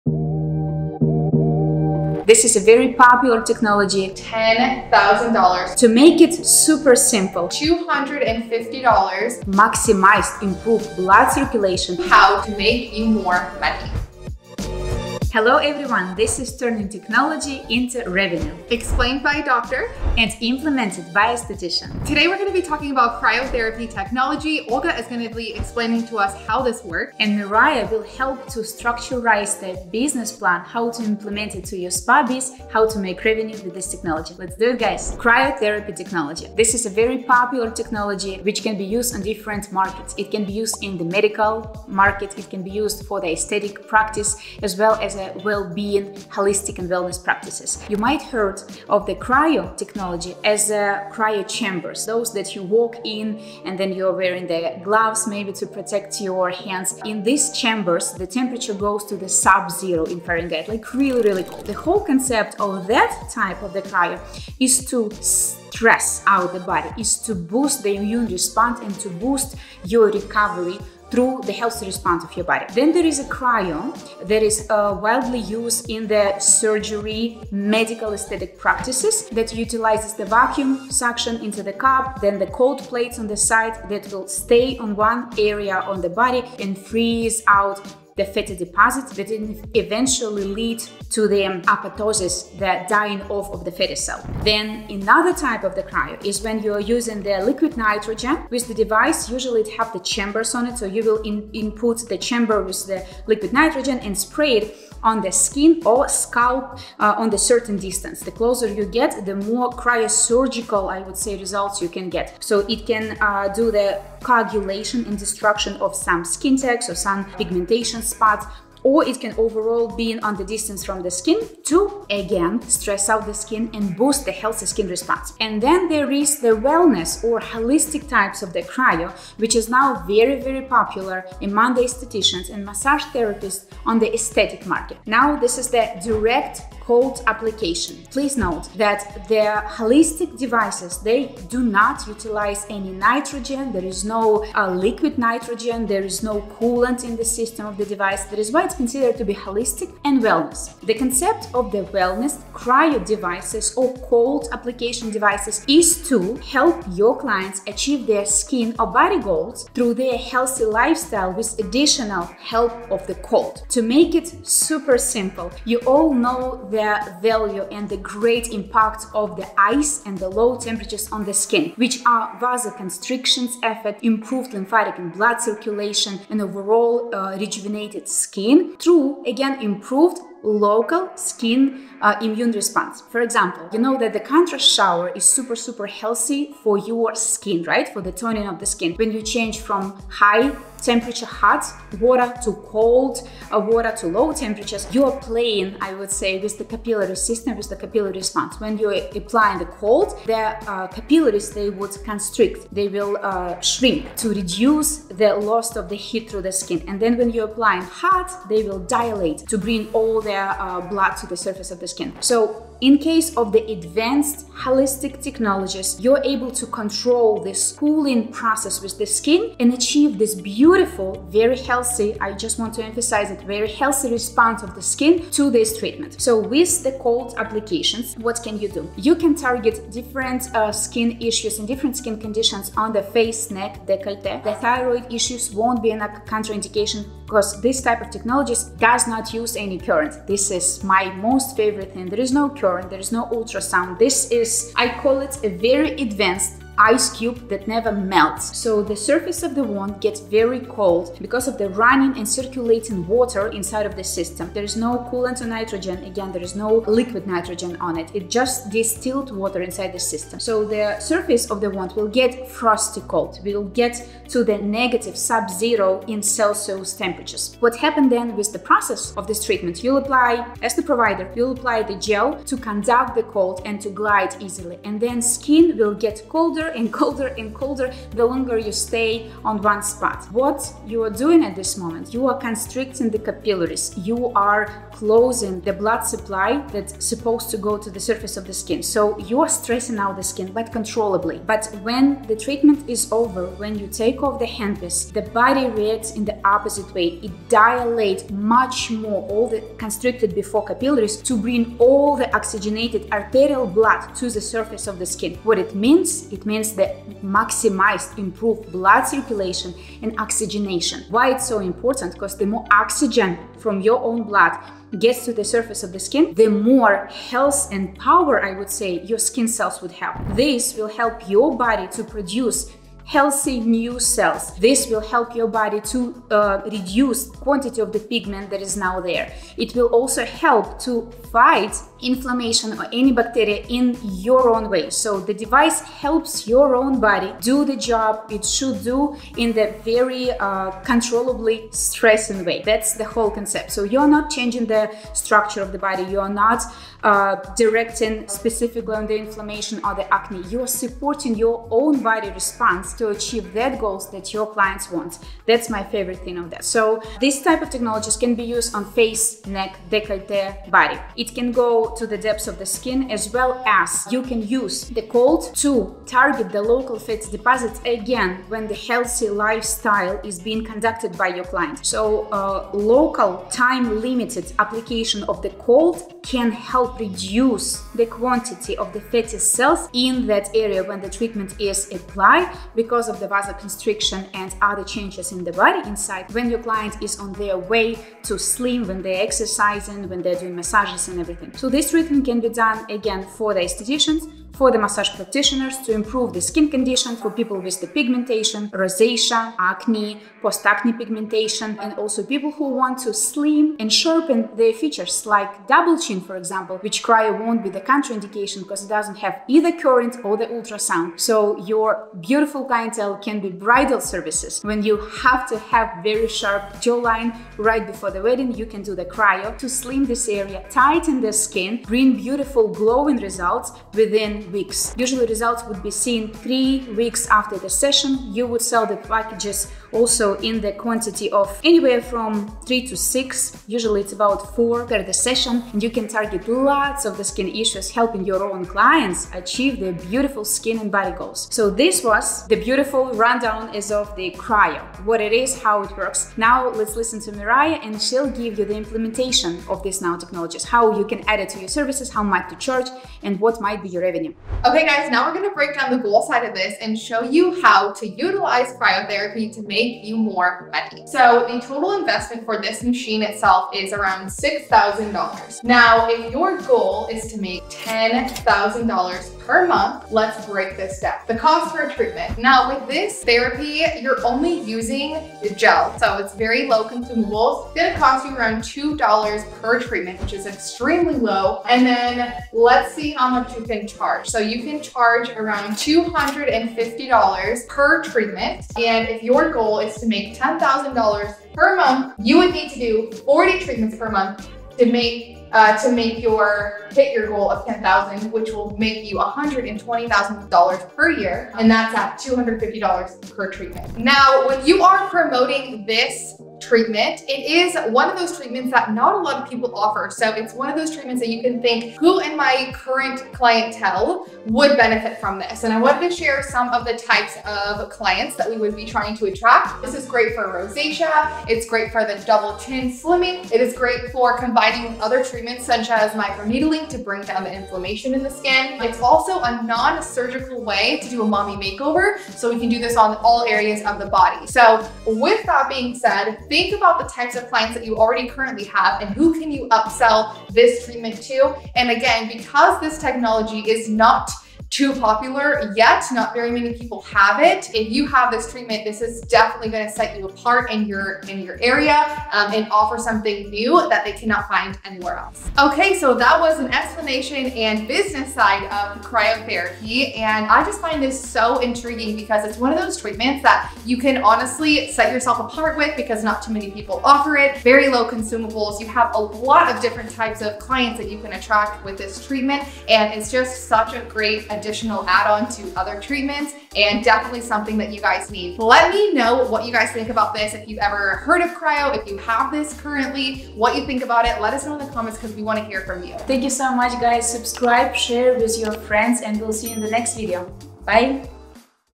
Cryo Therapy | Explained by Doctor and Implemented by Esthetician
Welcome to our latest video where Olha Maikina, MD, co-founder of Zemits, dives into the science and technology behind cryo therapy, detailing how it works and its incredible medical benefits. Joining her is Mariah Sanchez, Master Esthetician and founder of Project Esty, who shares practical tips on how estheticians can integrate this advanced technology into their services to enhance treatments and boost profits.

What You’ll Learn:

 1. Olha Maikina’s Expert Insight: Discover the science behind cryo therapy and its benefits from a medical perspective.
 2. Mariah Sanchez’s Practical Application: Learn how to implement cryo therapy in your esthetic practice and elevate your services.

Don’t miss out on this insightful discussion that can transform your practice with the latest in cryo technology.

Watch the video now and elevate your practice!